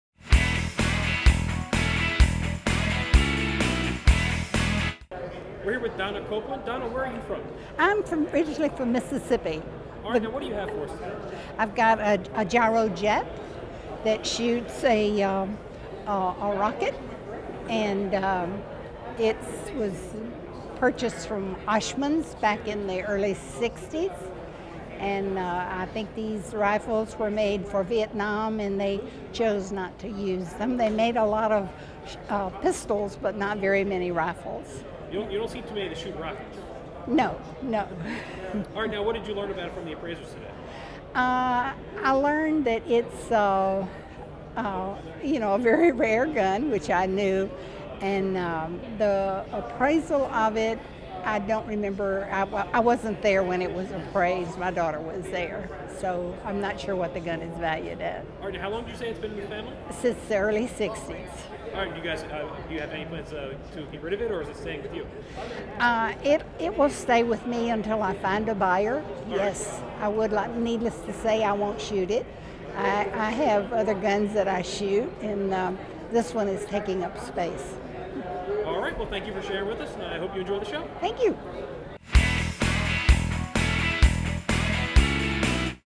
2013 NRA Annual Meetings: Antique Guns & Gold Showcase
NRA Members from around the nation brought firearms from throughout history to the Antique Guns & Gold showcase at the 142nd NRA Annual Meetings & Exhibits.